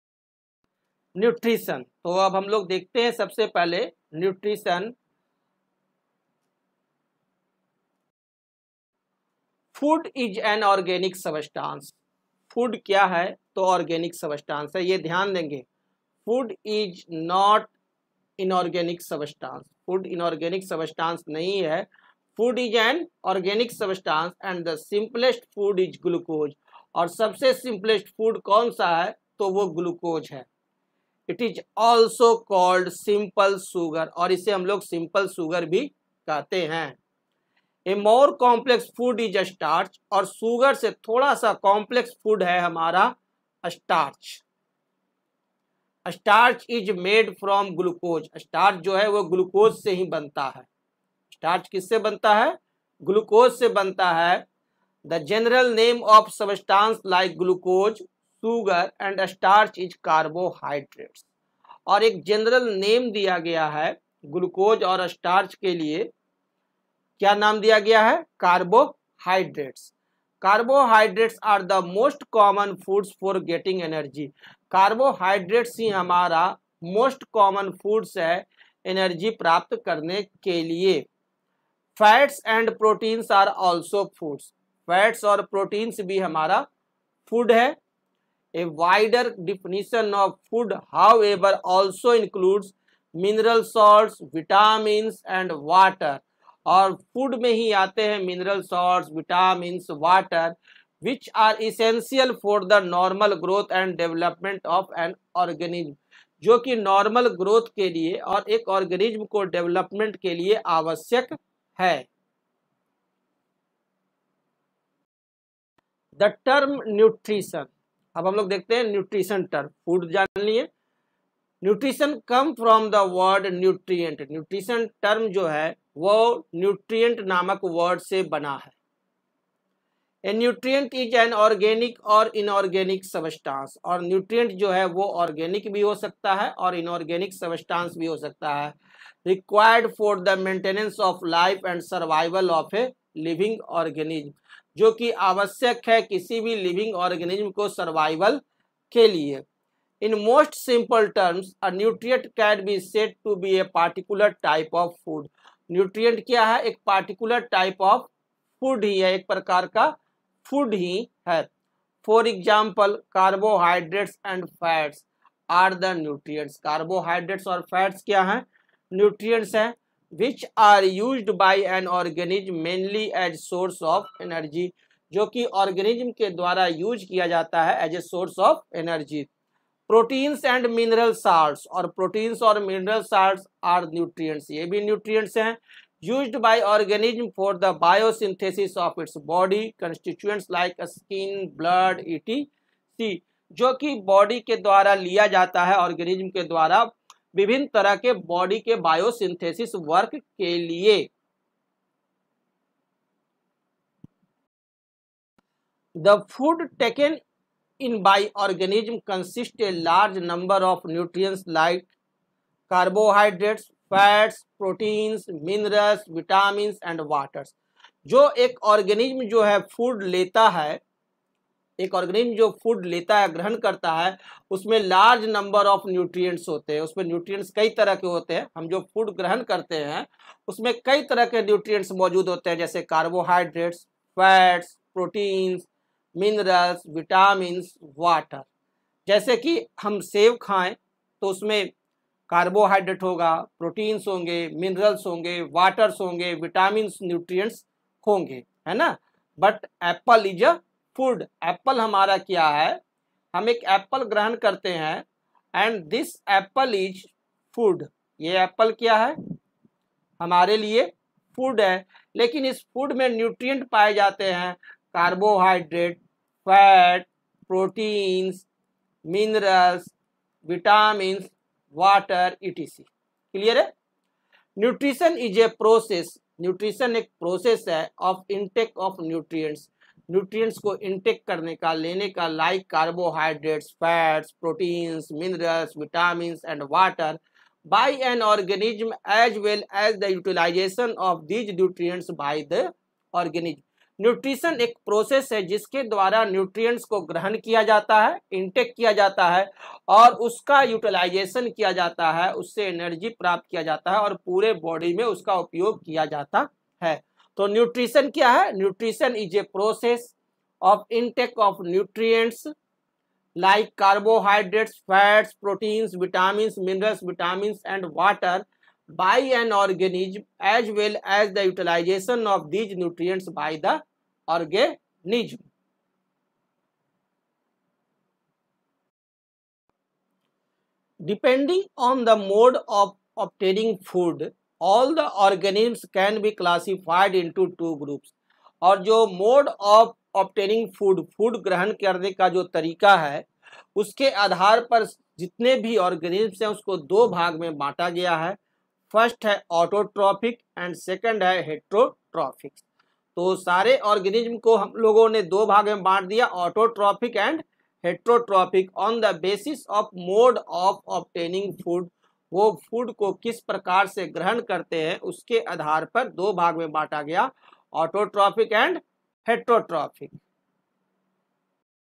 02 Nutrition | Life Process CBSE Class 10 Science Biology || life processes : nutrition 2
02 Nutrition | Life Process CBSE Class 10 Science (Biology) || NCERT | CBSE

Life Processes : Nutrition 2  ||  CBSE 10 BIOLOGY

The food taken in by an organism contains a large number of nutrients like carbohydrates, fats, proteins, minerals, vitamins and water, etc.
Nutrition is a process of intake of nutrients (like carbohydrates, fats, proteins, minerals, vitamins and water) by an organism as well as the utilisation of these nutrients by the organism.

Depending on the mode (or method) of obtaining food, all the organisms can be classified into two groups:
1. Autotrophic and
2. Heterotrophic.